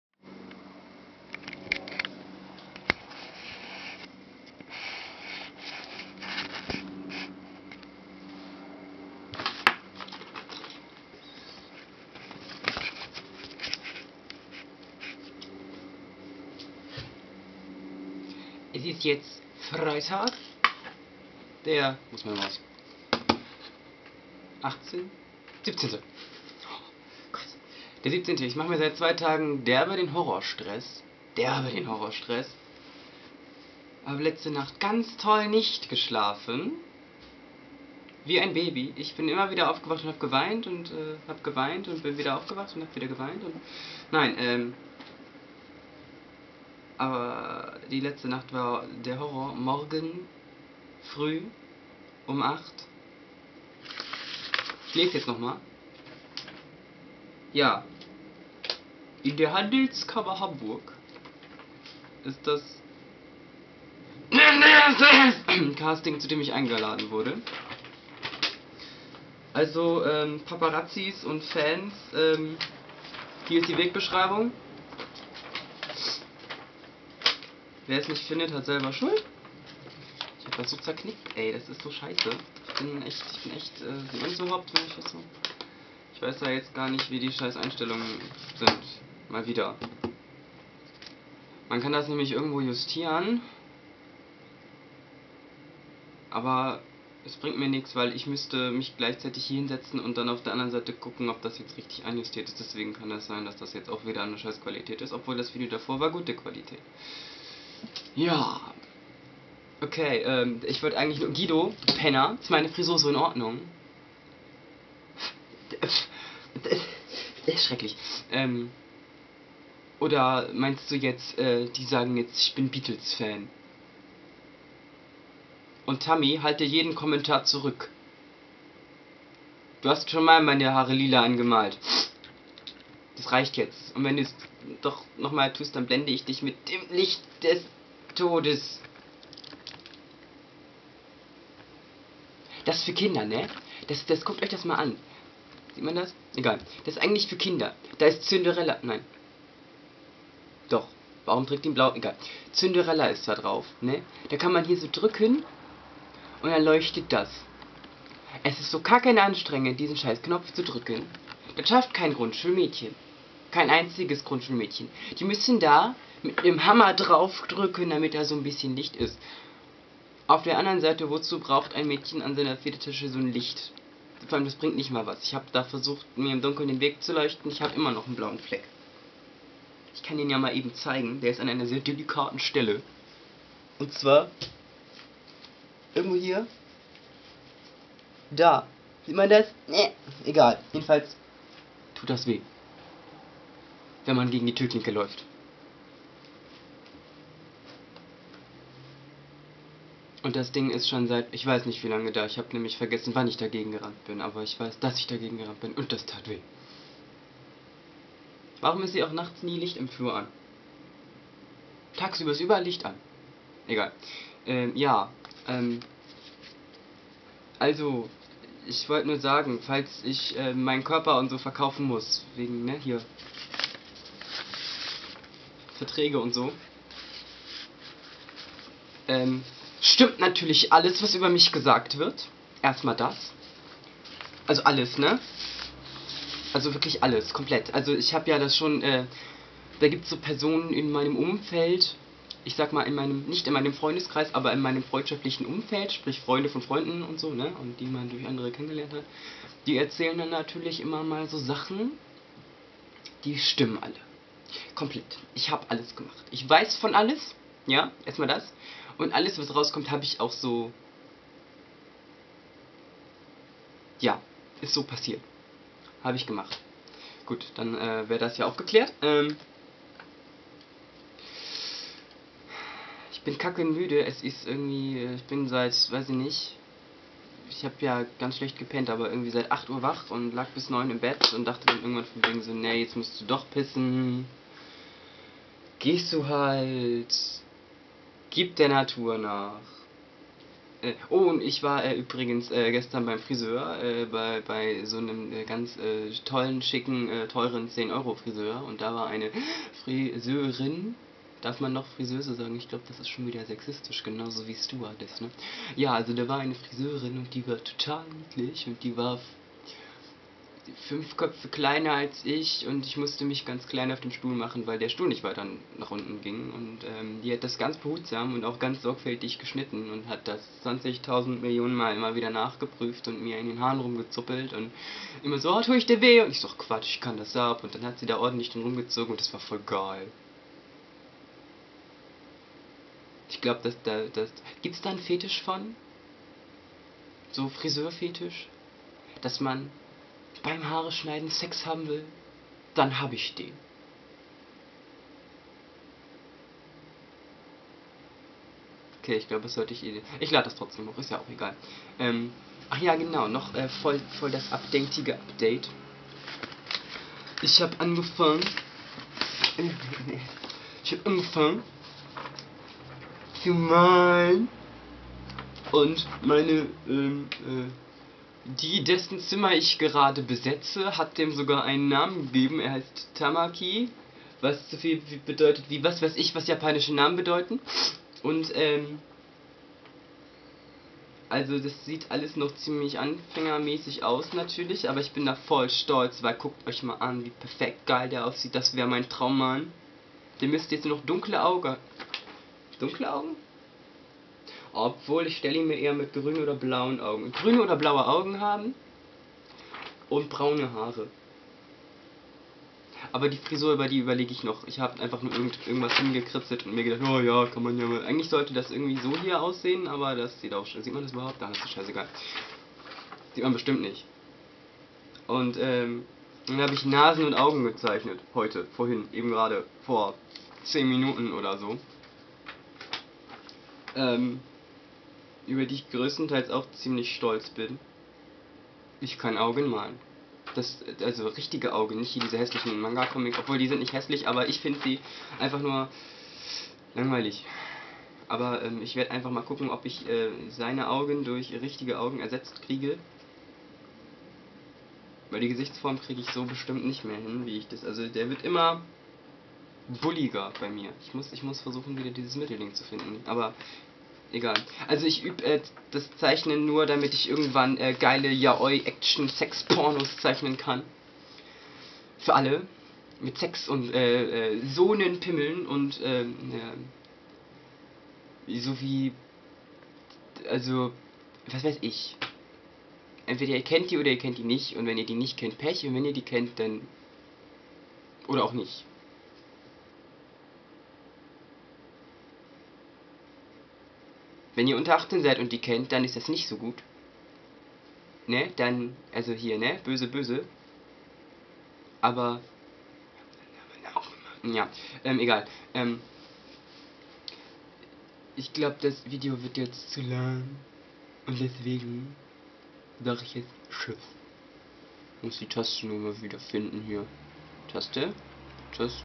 V-log mit Anfassen
Ja... Ihr habt NICHT unsubscribed, also gibts so lange mehr, bis ihr es tut.

Casting ist MORGEN (Samstag den 18. September)

Ich zeichnö Yaoi-Hardcore-Pr0n um damit alle japanophilen Anime-Manga-Wichser zu beglücken =D

KEIN KOMMENTAR ÜBER MEINE HAARE ES SEI DENN ICH HAB EUCH DRUM GEBETEN!!!

Nein, Guido ist NICHT der Westerwelle. Dessen Meinung interessiert mich diesbezüglich n Schaiß.

Ichweißimmernochnichwasichsingensollichverkacksowieso.